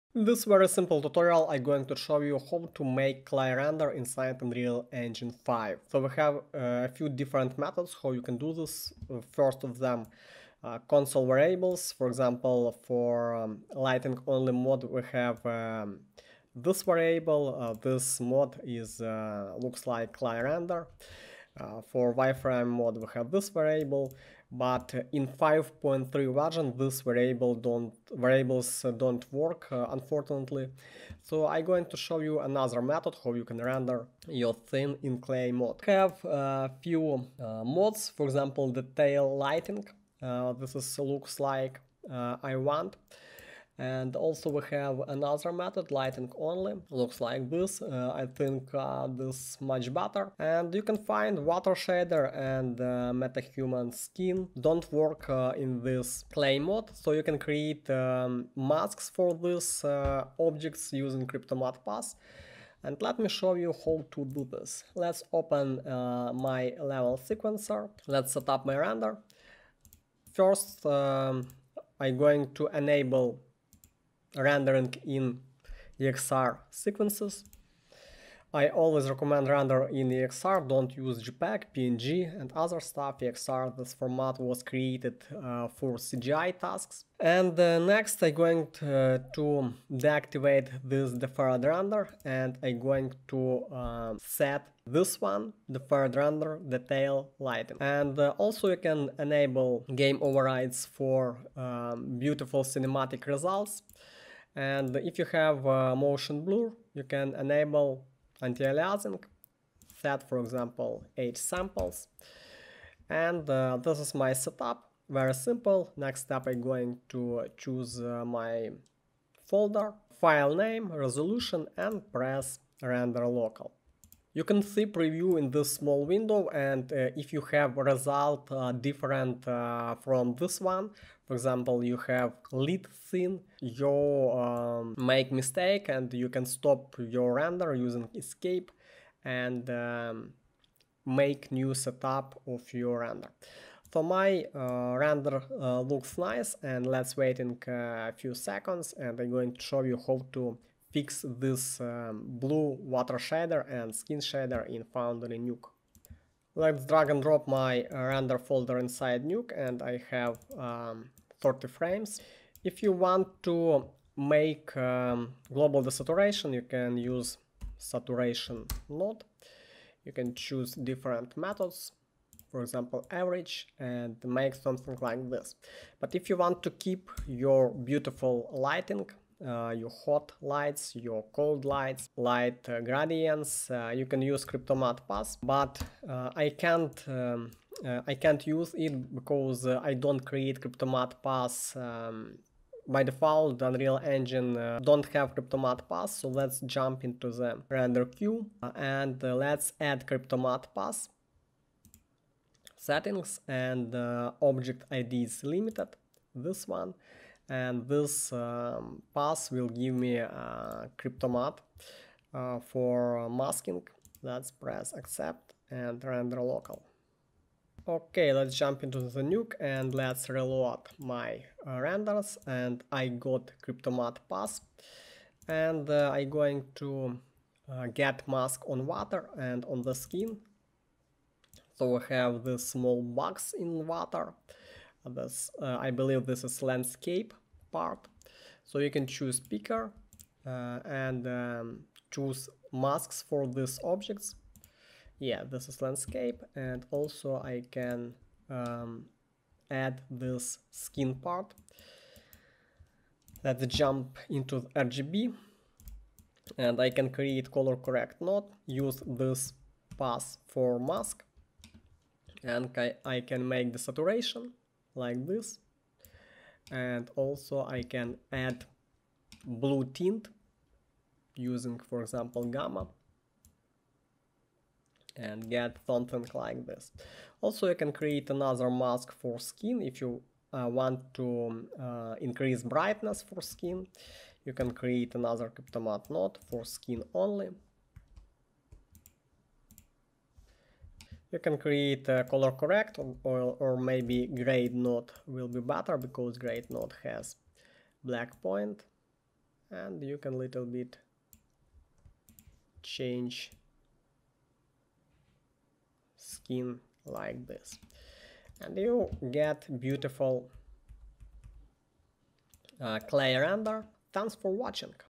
Clay Render - Unreal Engine 5
If you are looking for a fast way to easily override all materials in to a midtone grey. In this tutorial I going to show you a few ways.

You can use console commands to switch view modes - 'viewmode lightingonly' or use Deferred Rendering in Movie Render Queue. Unreal Engine Override all Material. Clay rendering look.

A "Clay Render" in the context of Unreal Engine 5 refers to a type of rendering mode or visualization technique that displays 3D scenes using a simplified, monochromatic representation without any textures, lighting effects, or materials. The term "clay" is used because the resulting image often resembles a sculpture made of clay.

This rendering mode is particularly useful during the development and design phase of a project for a few reasons:

Focus on Form and Structure: By removing textures and complex lighting, a clay render allows artists and developers to focus on the fundamental forms and structures of 3D models. It provides a clear view of the geometry, helping to identify any issues related to mesh topology, proportions, or general shape.

Optimization and Performance: When working on a scene with a large number of detailed textures and complex materials, it can be computationally expensive to render in real-time. Switching to a clay render can be a quick way to optimize performance during the development process, especially when dealing with complex scenes.

Artistic Decision-Making: Artists and designers often use clay renders to assess the overall composition and layout of a scene without being influenced by the final visual aesthetics. This allows for more informed artistic decision-making in terms of camera angles, object placement, and scene composition.

Enabling a clay render mode in Unreal Engine 5 might involve adjusting the rendering settings in the editor. This can include toggling specific viewport modes, adjusting post-processing effects, or applying special materials to objects in the scene. Keep in mind that the exact method may vary based on the version of Unreal Engine and any updates or changes made to the software.

Using clay renders is a common practice in 3D graphics and game development, and it's not exclusive to Unreal Engine. It serves as a valuable tool in the iterative process of refining and optimizing 3D content before applying final textures, lighting, and materials for the polished visual presentation of a project.